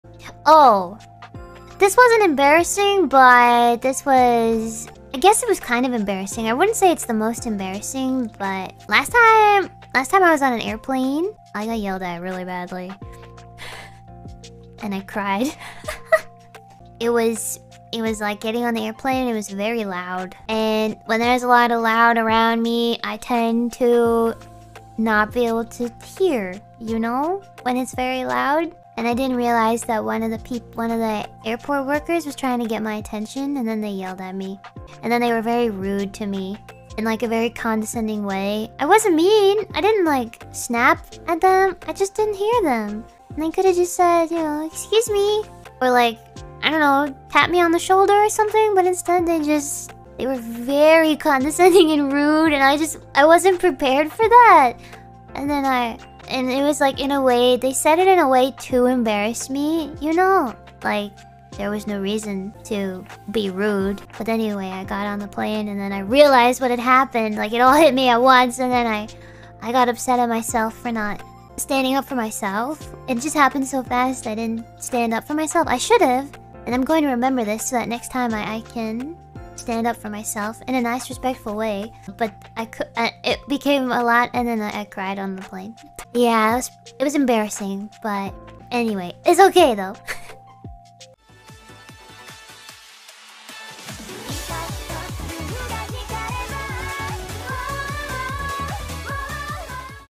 𝐆𝐮𝐫𝐚 𝐠𝐞𝐭𝐬 𝐲𝐞𝐥𝐥𝐞𝐝 𝐚𝐭 𝐛𝐲 𝐭𝐡𝐞 𝐀𝐢𝐫𝐩𝐥𝐚𝐧𝐞 𝐰𝐨𝐫𝐤𝐞𝐫 |Hololive EN
ED ART by @Tkminnm from Twitter -

BGM used
DOVA-SYNDROME